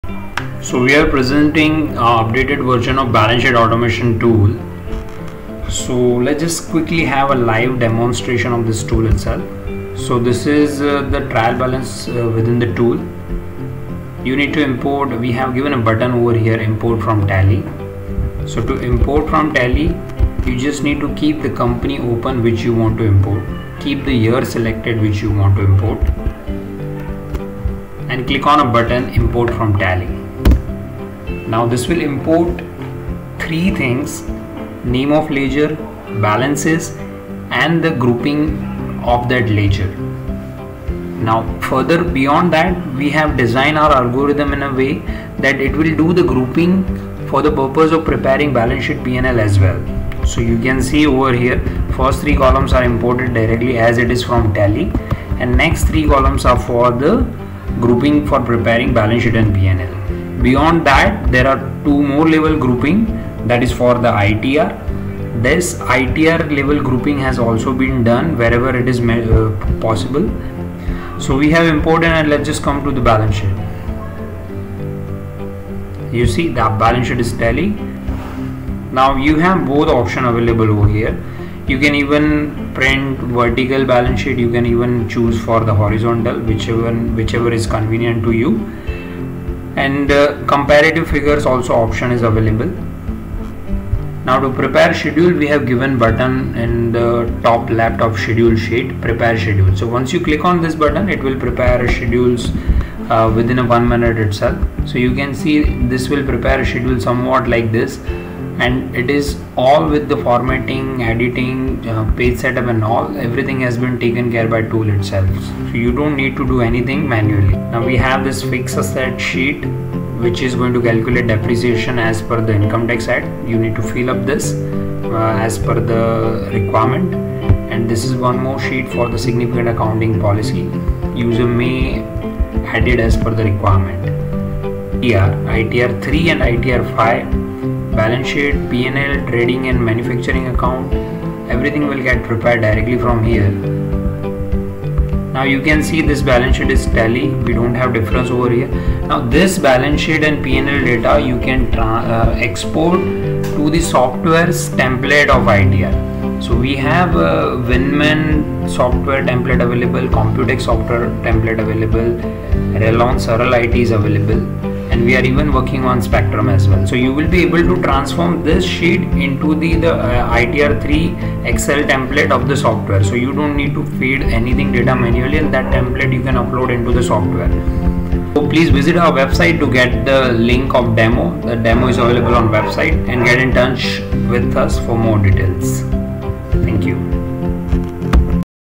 Balance Sheet Automation Tool- Live Demo || Tally to Excel || ITR 3 & 5 Balance Sheet, P&L
This is  Excel Automation tool for CA firms. Developed by CA for CAs.

This is live demonstration of Balance Sheet Automation Tool to Prepare Balance Sheet, Trading Account, Profit and Loss Account along with Schedules and ITR 3 /  5 Balance sheet, Trading, P&L Account.

Key Features
 - Prepares ITR 3 & 5 Balance Sheet, Profit & Loss A/c, Trading/Manufacturing A/c
 - Branch Consolidation of Accounts
 - Columnar Balance Sheet, P&L, Elimination Report
 - Import multiple tally companies into single excel file
 - 8 Colour Themes of Financials
 - Auto Grouping
 - Auto Page Setup

Team,
Black Horse Excellence